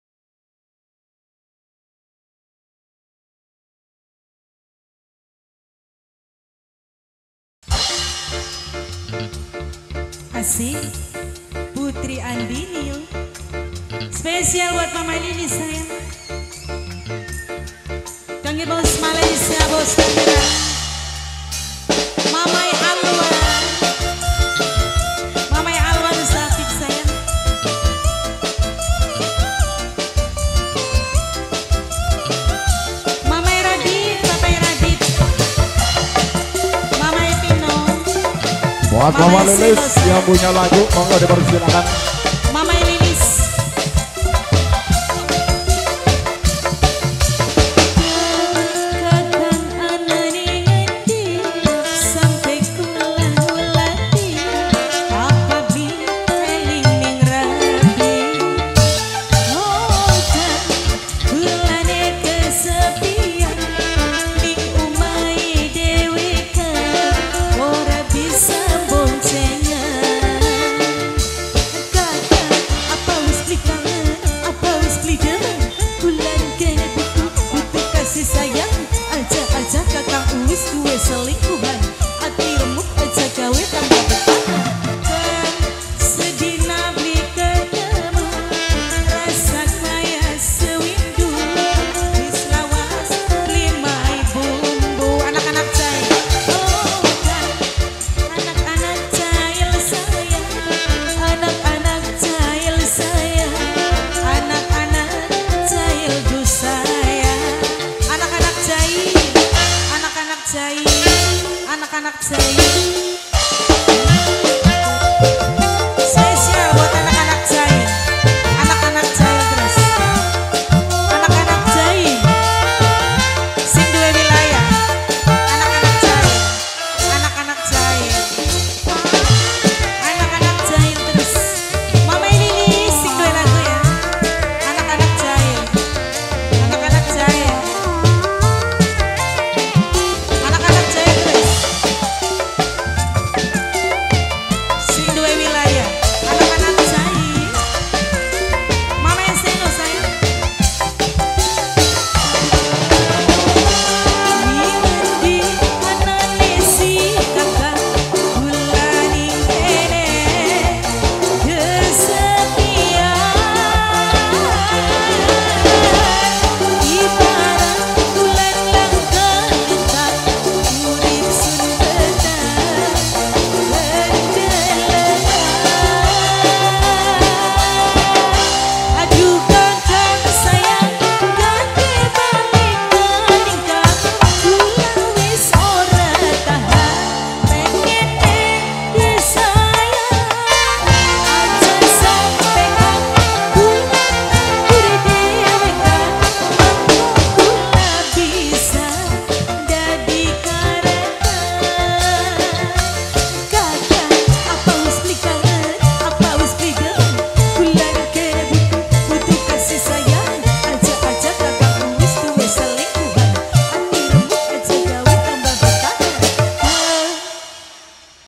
16. PUTRI ANDINI TUMARITIS BLOK 2 13 AGUSTUS PURNOMO NURYATI-AFIS STUDIO
PESTA HAJAT Kel. Bp. PURNOMO - Ibu NURYATI. Selasa, 13 Agustus 2019. Desa Karangtumaritis Blok 2 Kec. Haurgeulis - Kab. Indramayu. Khitanan: PUTRA BAGUS